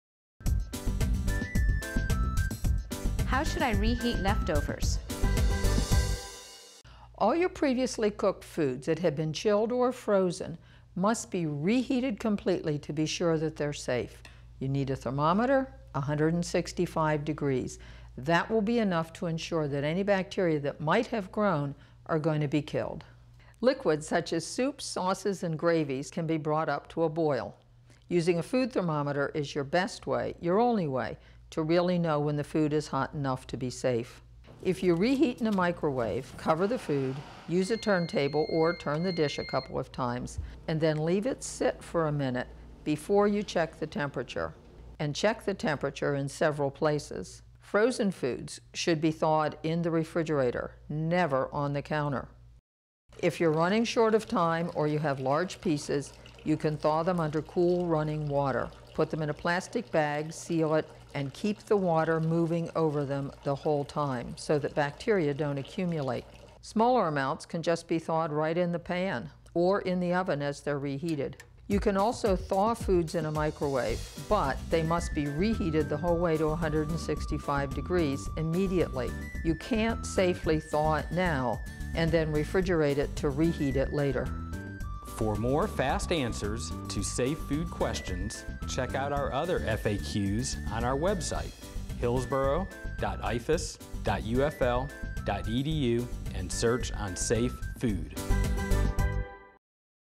Safe Food FAQs - How Should I Reheat Leftovers?
Leftovers that are not reheated safely can be dangerous.  Learn what the safe temperature is for reheating foods, 165° F, how to reheat safely, and how to thaw foods safely prior to reheating.
Producer and Scriptwriter, Mary Keith; Videographer, Graphics and Editing, Billie Lofland; County Extension Director, Stephen Gran.